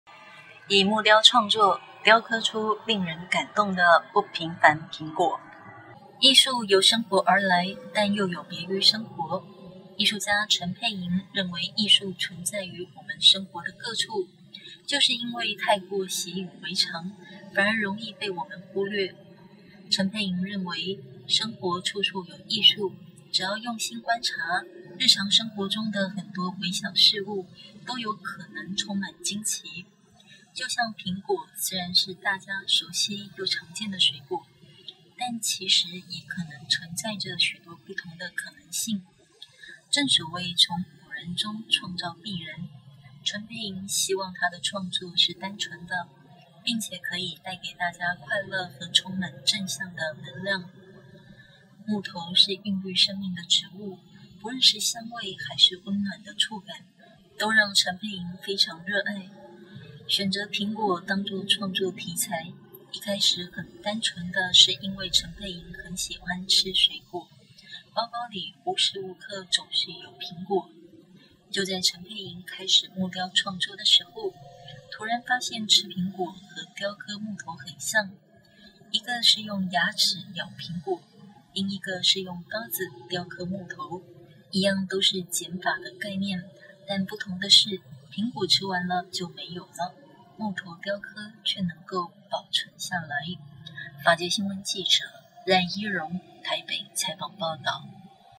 法界新聞》20151201蘋凡中的不平凡 陳珮吟木雕展
法界新聞》
以木雕創作
雕刻出
令人感動的
不平凡蘋果